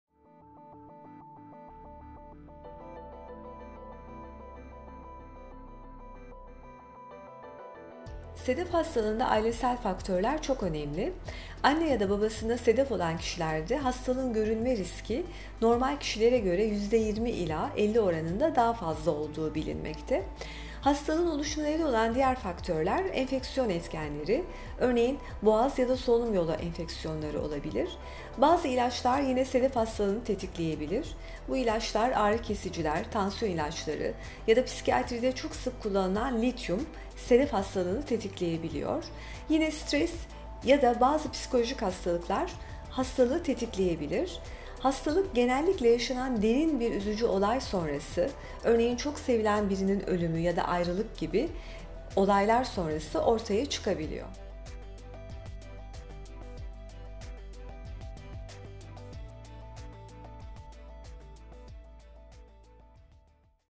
Sedef Hastalığı Neden Ortaya Çıkar - Prof. Dr. Gonca Gökdemir
Prof. Dr. Gonca Gökdemir "Sedef Hastalığı Neden Ortaya Çıkar?" sorusunun cevabını veriyor.